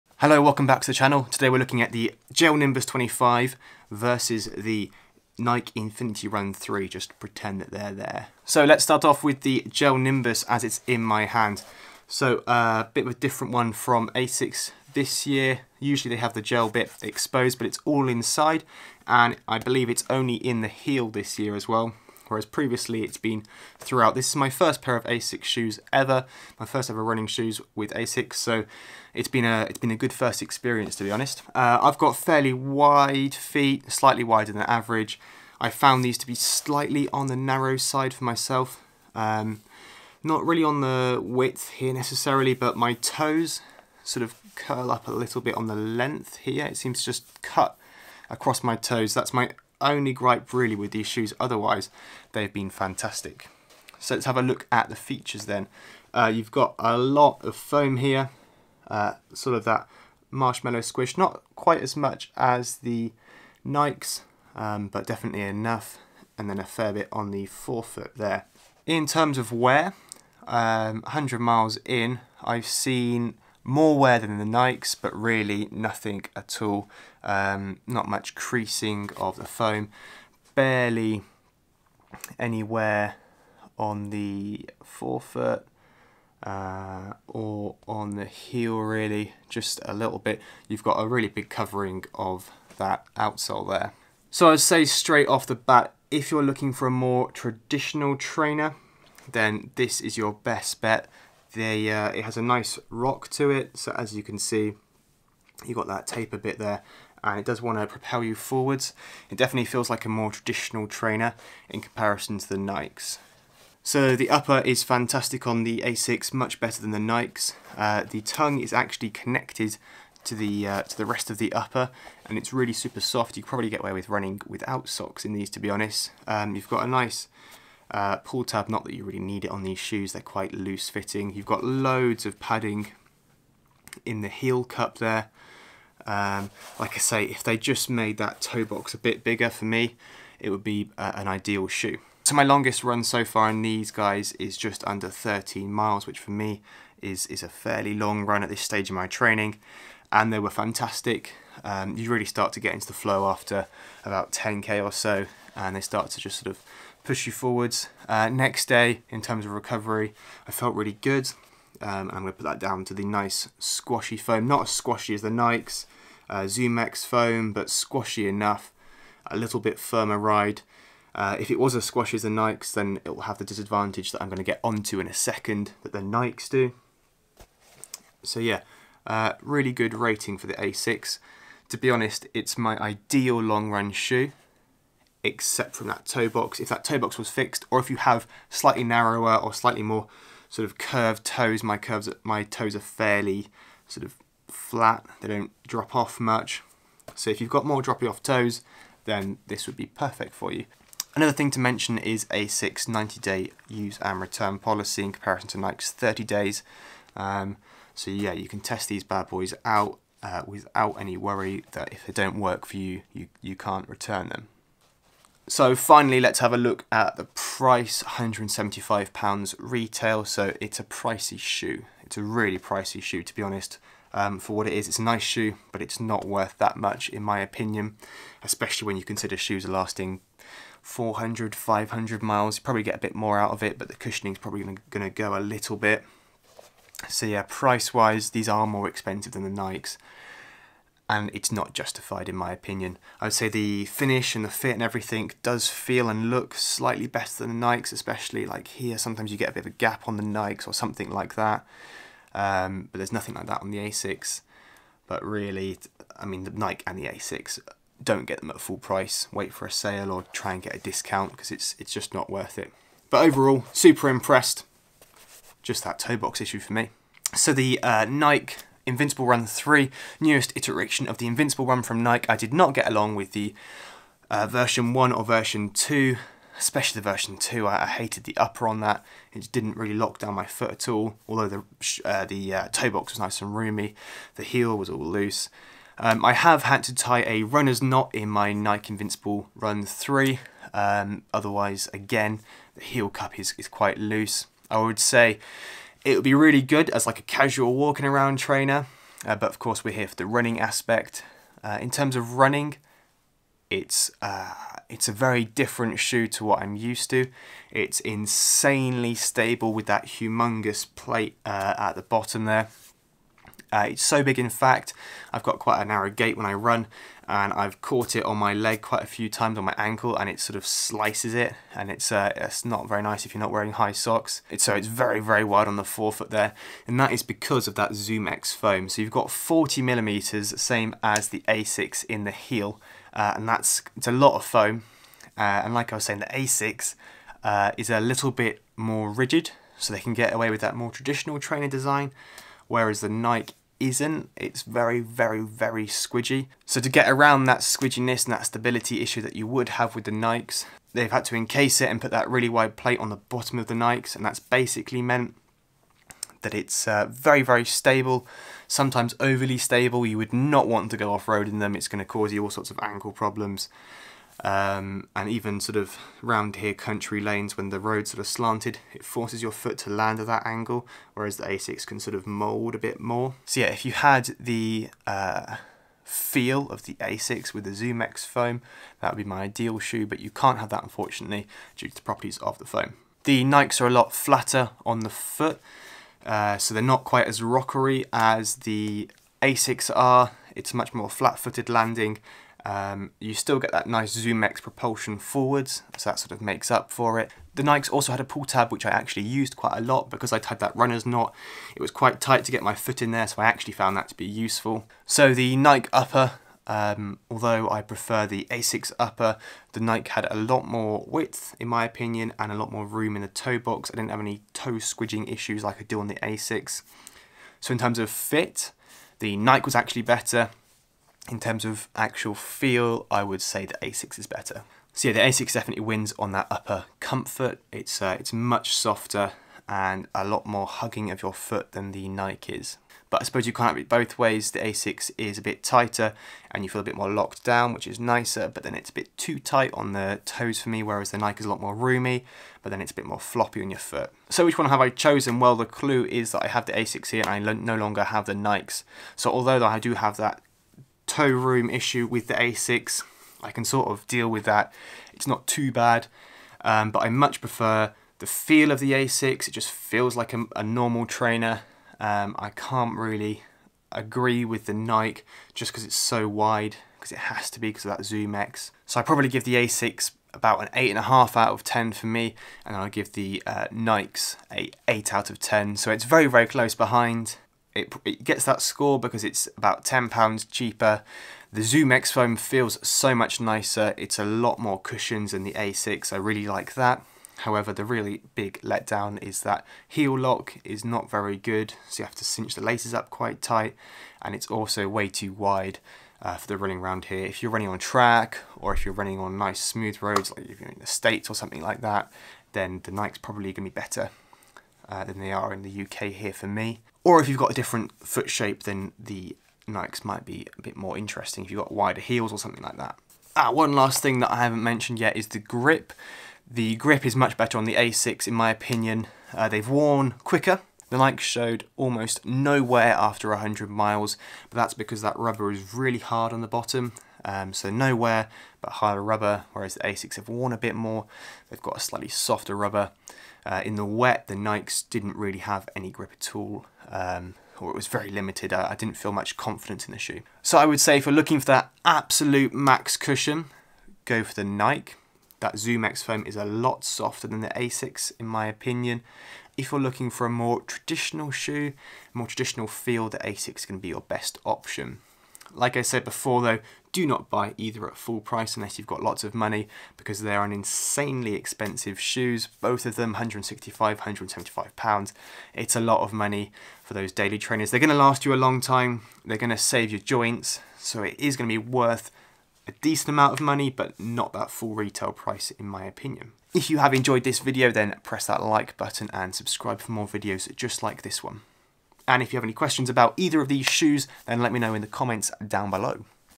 Asics gel nimbus 25 VS Nike Invincible run 3 | Which is the best running shoe?
Nike Invincible 3 or Asics Gel Nimbus 25? These shoes definitely both have their ups and downs but the Nike Invincible 3 just isn't quite as good as the Asics Gel Nimbus 25 in my opinion... but that being said, it completely depends on foot shape! Might be worth giving both the Asics Gel Nimbus 25 and the Nike Invincible 3 a go to see for yourself...good luck!